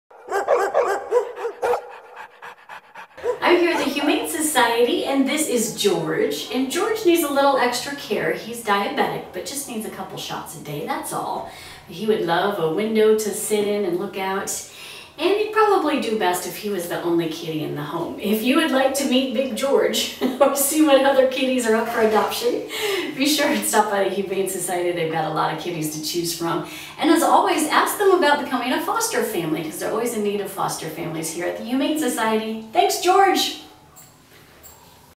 Page's Perfect Pet - George
George would like a forever home with a window seat! Find him at the Humane Society of Southwest Missouri.